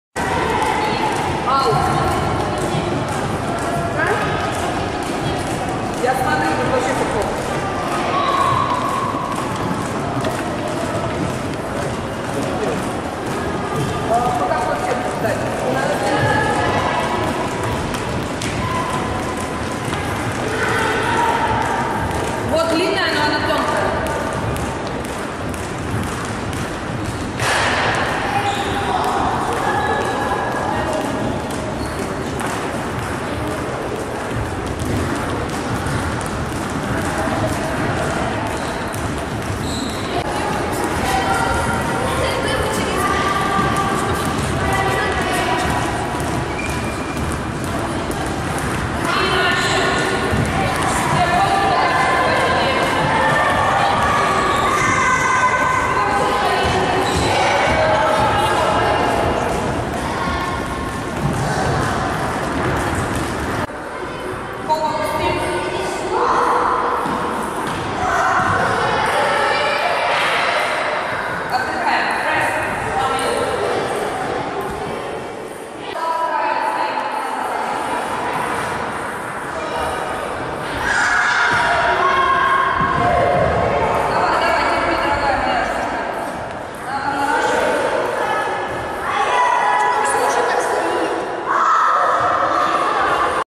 Wednesday morning physical training in Narva summer camp (8.6.2016)
Here are some flashes about physical training after Wednesday morning's table tennis training.